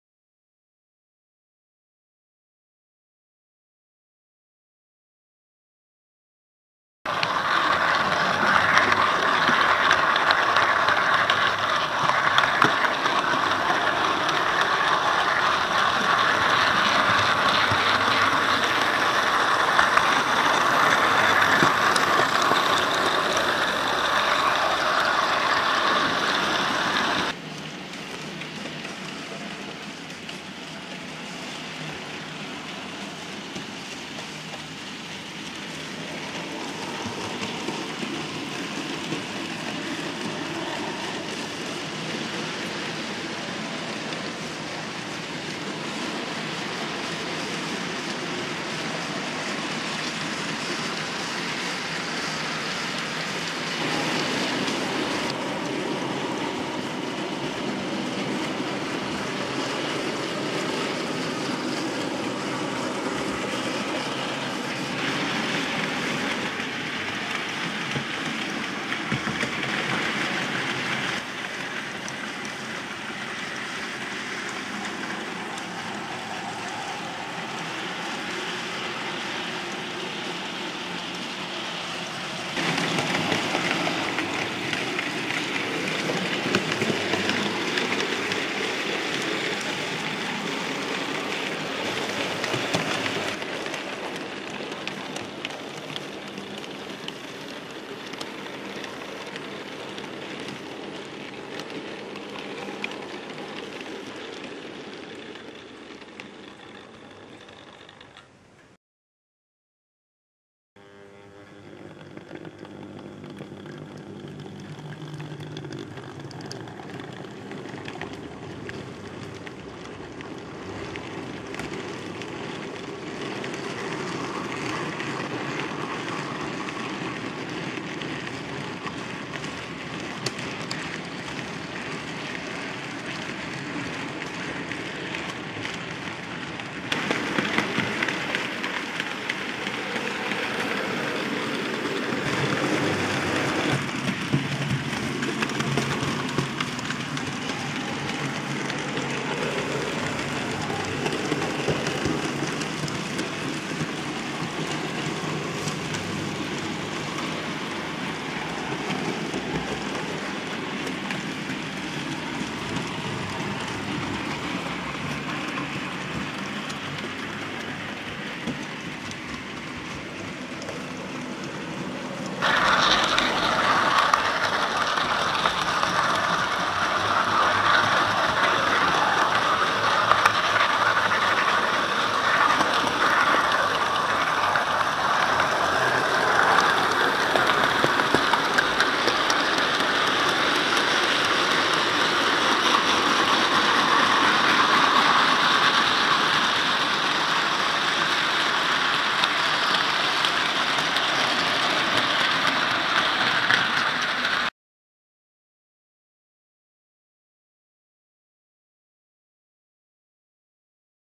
From The Lineside Number 269 LMS Locomotive Ratings 2F
Bachmann's duo of 2-6-0 Class 2 locomotives haul a pair of Goods trains about the layout.